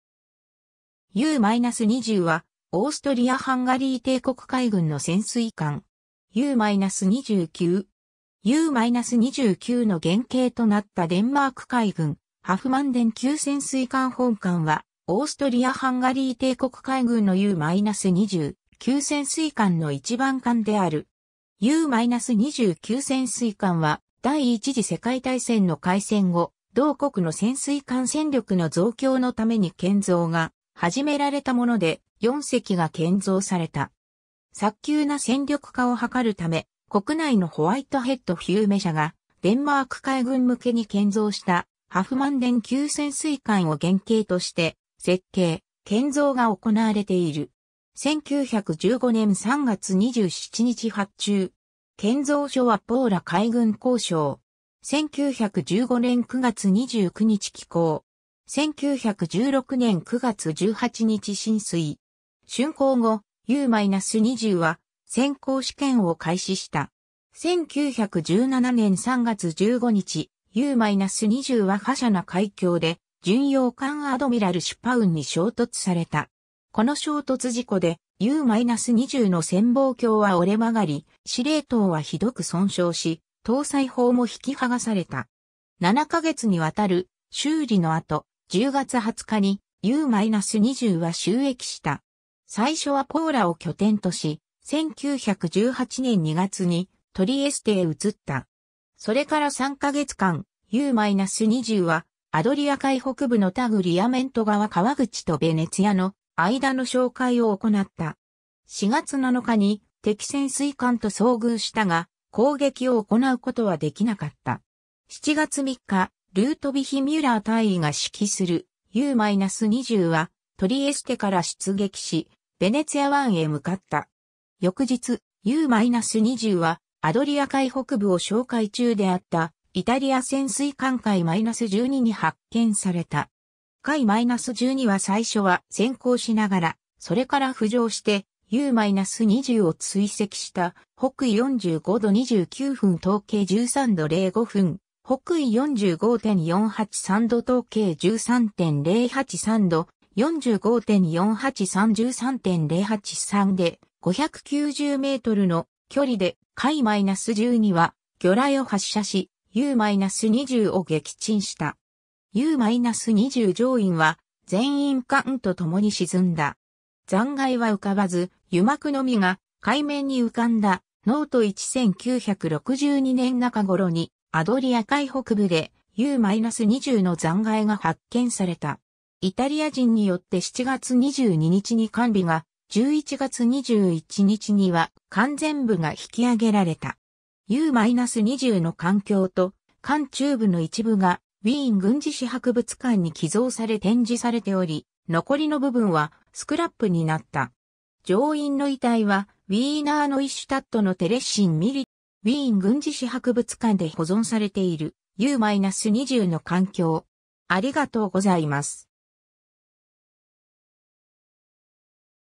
U-20 (オーストリア＝ハンガリー帝国潜水艦)
オーストリア＝ハンガリー帝国の潜水艦
U-20（SM U-20またはU-XX）はオーストリア＝ハンガリー帝国海軍の潜水艦。
U-20級。
 U-20級の原型となったデンマーク海軍ハフマンデン級潜水艦 本艦は、オーストリア＝ハンガリー帝国海軍のU-20級潜水艦の1番艦である。
U-20級潜水艦は、第一次世界大戦の開戦後、同国の潜水艦戦力の増強のために建造が始められたもので、4隻が建造された。
早急な戦力化を図るため、国内のホワイトヘッド・フューメ社がデンマーク海軍向けに建造したハフマンデン級潜水艦（英語版）を原型として設計・建造が行われている。
 1915年3月27日発注。
建造所はポーラ海軍工廠。
1915年9月29日起工。
1916年9月18日進水。
 竣工後、U-20は潜航試験を開始した。
1917年3月15日、U-20はFasana海峡で巡洋艦アドミラル・シュパウンに衝突された。
この衝突事故でU-20の潜望鏡は折れ曲がり、司令塔はひどく損傷し、搭載砲も引き剥がされた。
7ヶ月に亘る修理の後、10月20日にU-20は就役した。
 最初はポーラを拠点とし、1918年2月にトリエステへ移った。
それから3ヶ月間、U-20はアドリア海北部のタグリアメント川河口とヴェネツィアの間の哨戒をおこなった。
4月7日に敵潜水艦と遭遇したが、攻撃を行うことは出来なかった。
 7月3日、ルートヴィヒ・ミュラー (Ludwig Müller) 大尉が指揮するU-20はトリエステから出撃しヴェネツィア湾へ向かった。
翌日、U-20はアドリア海北部を哨戒中であったイタリア潜水艦F-12に発見された。
F-12は最初は潜航しながら、それから浮上してU-20を追跡した北緯45度29分 東経13度05分 / 北緯45.483度 東経13.083度 / 45.483; 13.083で、590mの距離でF-12は魚雷を発射し、U-20を撃沈した。
U-20乗員は全員艦と共に沈んだ。
残骸は浮かばず、油膜のみが海面に浮かんだ[Note 1] 1962年中頃にアドリア海北部でU-20の残骸が発見された。
イタリア人によって7月22日に艦尾が、11月21日には艦前部が引き揚げられた。
U-20の艦橋と艦中部の一部がウィーン軍事史博物館に寄贈され展示されており、残りの部分はスクラップになった。
乗員の遺体はウィーナー・ノイシュタットのTheresian Military Academyに埋葬された。
 ウィーン軍事史博物館で保存されているU-20の艦橋